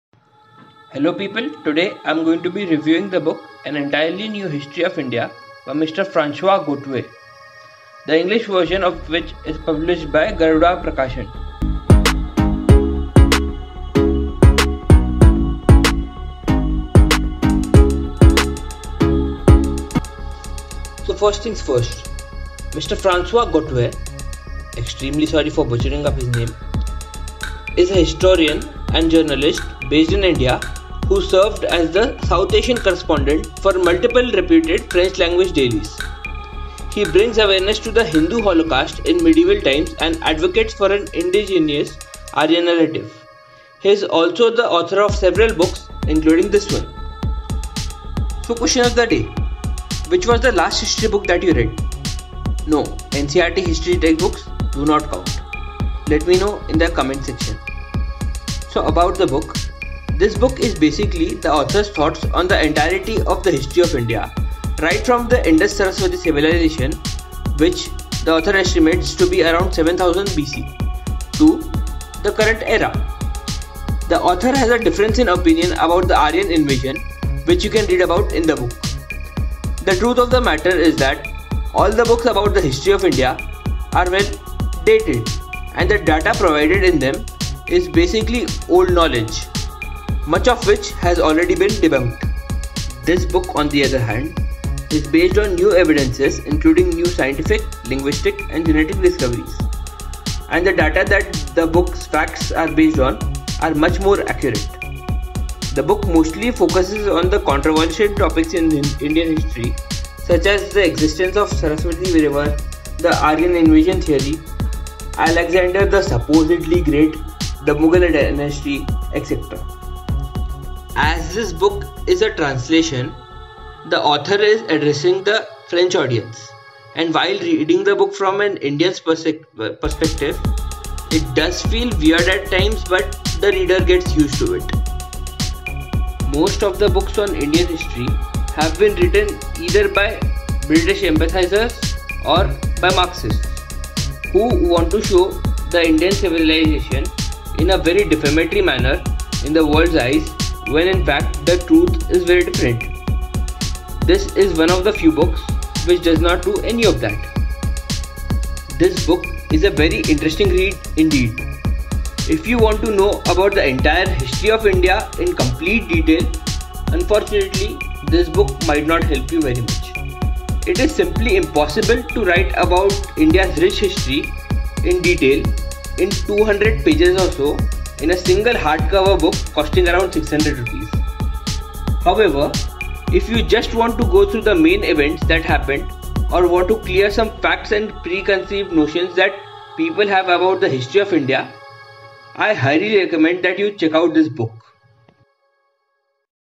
MY REVIEW OF "AN ENTIRELY NEW HISTORY OF INDIA" BY FRANCOIS GAUTIER
I received a review copy of this book from Garuda Prakashan in exchange of an honest review.

Hardcover Best Buy Link

If you're the Tiktok Type , then perhaps you're not my Type